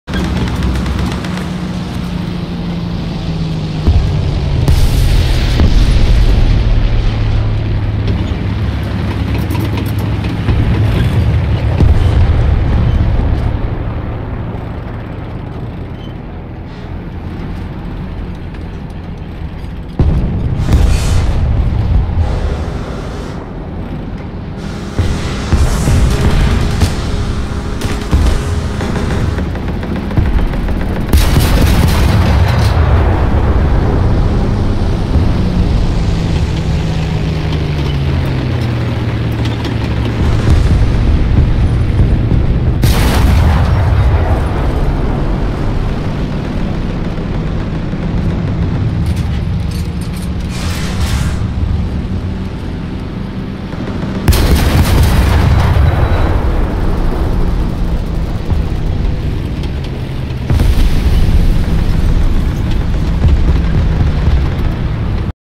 20180903-233137 RB-RU ZUT-37
This was my second shooting down in the ZUT-37. The first was more spectacular :)
The vehicle is one of my best purchases for a personal discount (50% of GE).